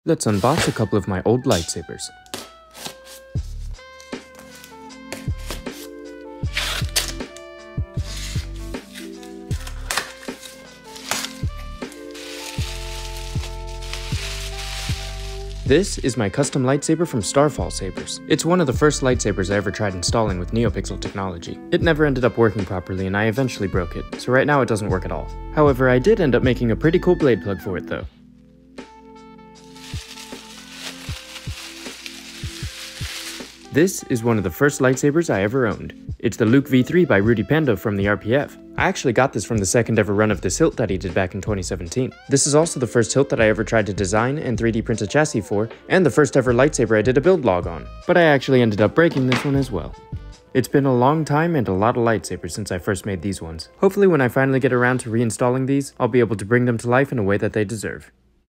Unboxing My Old Lightsabers | Luke V3 + Starfall Custom
This is the full width unboxing video of my old Luke V3 Lightsaber and Starfall Custom Saber. It’s another short length video because it’s originally made for tiktok/youtube shorts, but since it’s filmed landscape I wanted to post it this way as well. Hope you enjoy!

Where to buy these lightsaber hilts:

Luke V3: Rudy Pando (Spirit Of Obi1) on TheRPF

Starfall Custom Sabers:

Music in this video is used with the permission of Closed On Sunday: